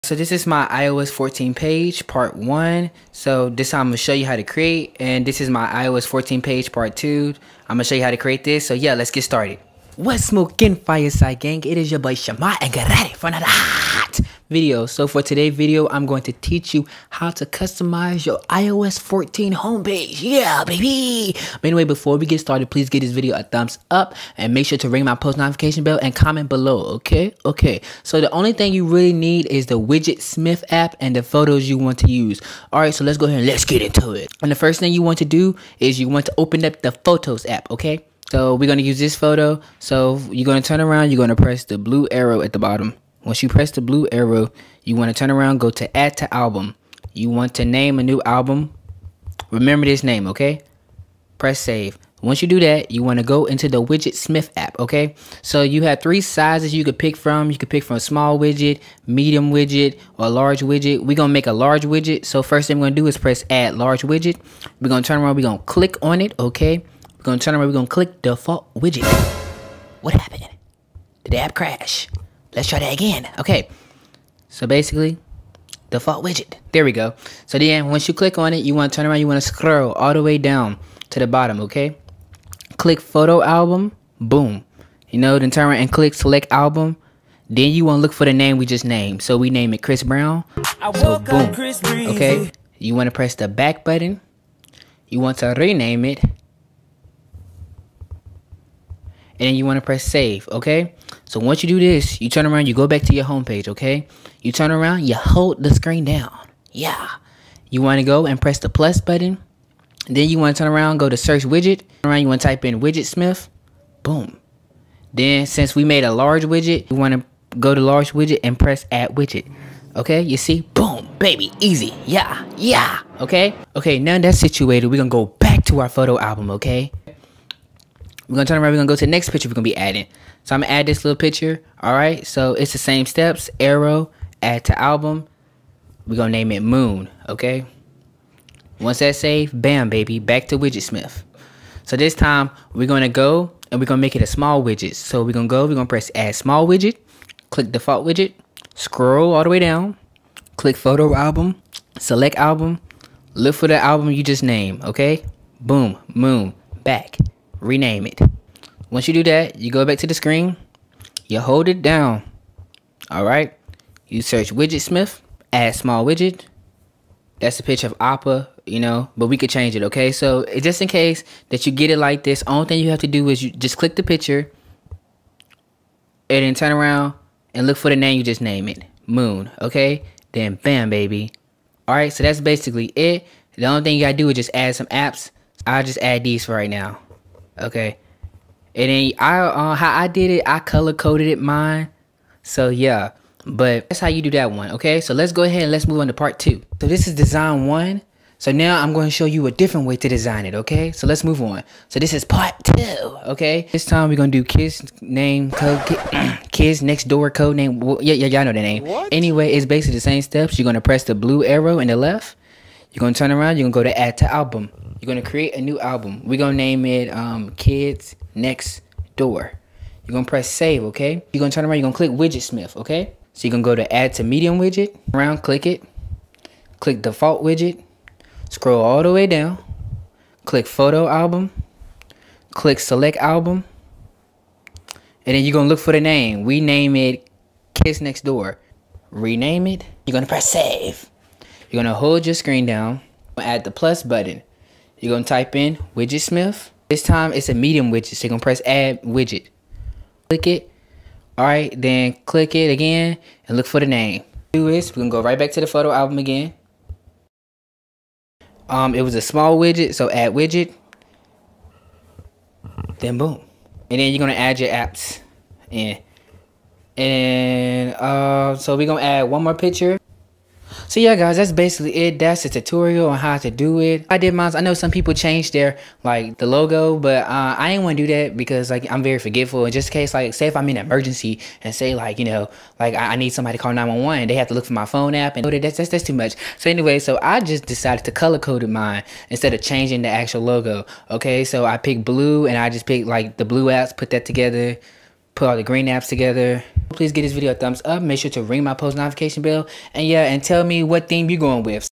HOW TO ADD PHOTOS USING WidgetSmith TUTORIAL (IOS 14 Home Screen Customization)
Learn  how to customize your ios 14 Home Screen and homepage  by adding  widgets. I will also show you my ios 14 Home Screen  so you can get some ideas

 I suggest picking a theme for your IOS 14 Home Screen. the themes I used with Billie Eilish and Avatar.

The only app  you will need is WidgetSmith.